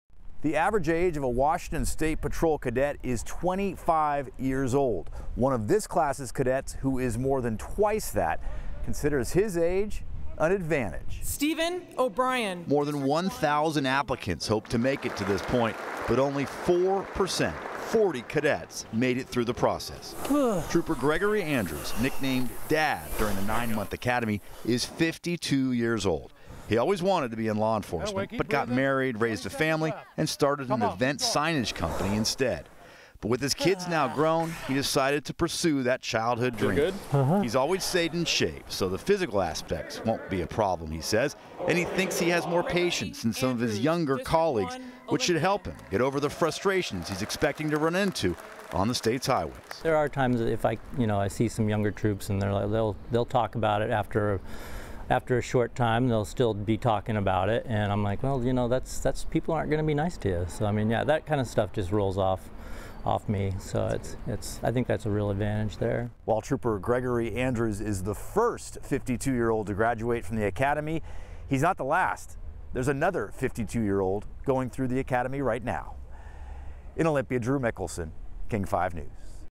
52-year-old WSP cadet graduates
A new class of state troopers is ready to hit the road and one has a lot more life experience than the rest.